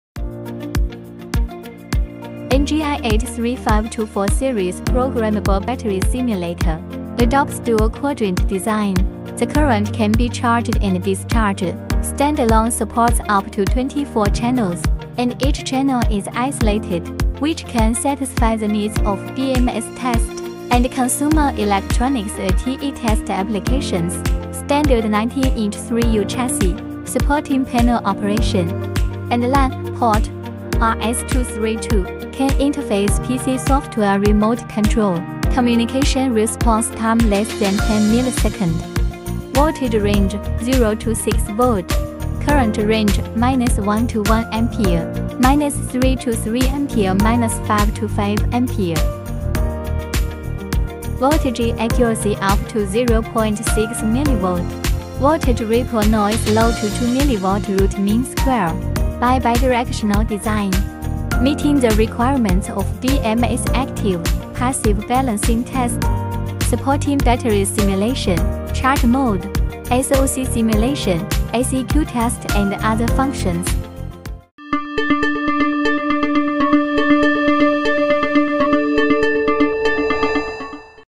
NGI N83524 24 channel Dual quadrant Battery Simulator for testing BMS using passive or active
N83524 is a programmable battery simulator with low-power, multi-channel and high-accuracy. By adopting dual-quadrant design, the current can be charged and discharged, which can satisfy the needs of BMS test and consumer electronics ATE test. Its voltage accuracy is up to 0.6mV, supporting ¦ÌA-level current measurement, standalone up to 24 channels. The channels are isolated from each other, which is convenient for series connection. N83524 supports both local operation and remote operation via LAN/RS232/CAN interface. N83524 application software is easy to use, which can meet demands of battery simulators in multi-channel, multi-parameter, and complex test environments.